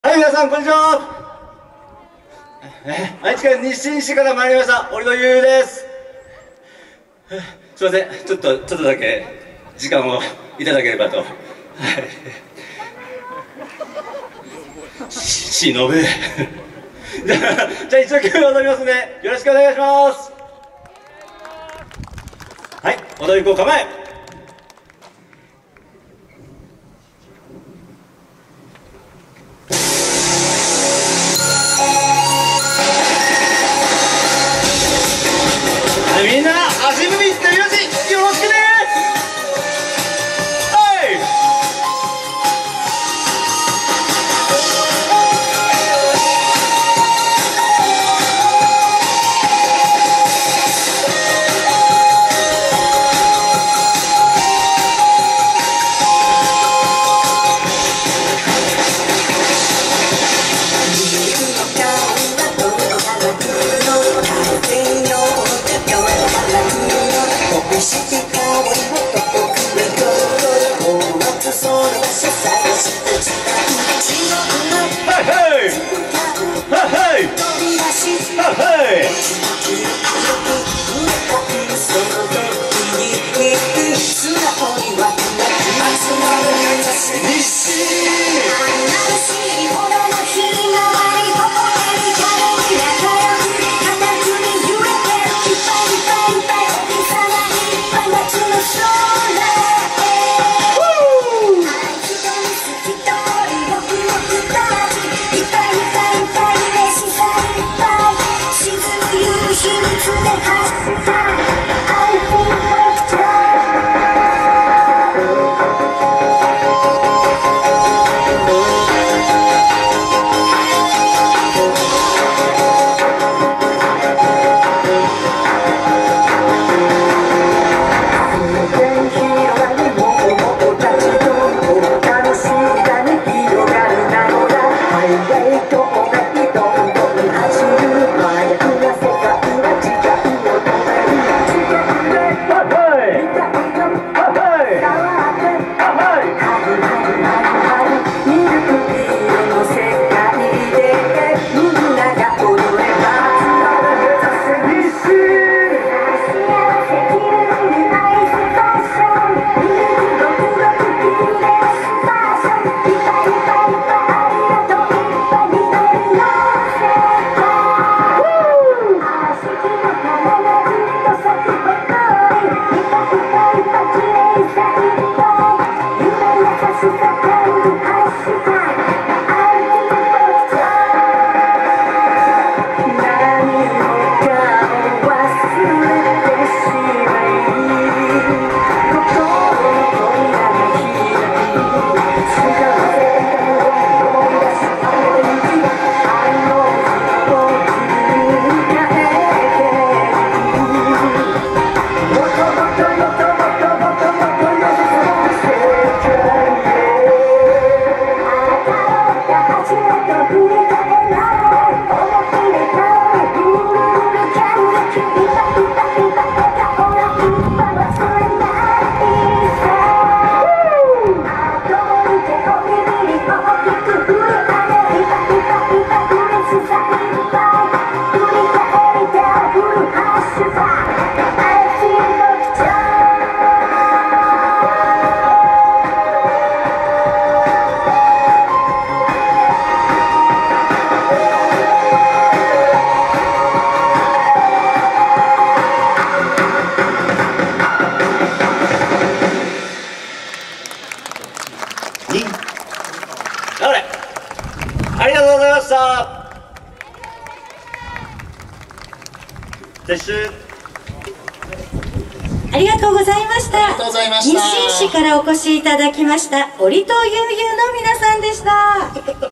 折戸悠悠 @ チャオ！茶の街西尾で踊っちゃお！！2021 (愛知こどもの国) 2021-05-09T13:41
2021年5月9日(日)、愛知県西尾市で開催された「チャオ！茶の街西尾で踊っちゃお！！2021」(愛知こどもの国)での「折戸悠悠」さんの演舞です。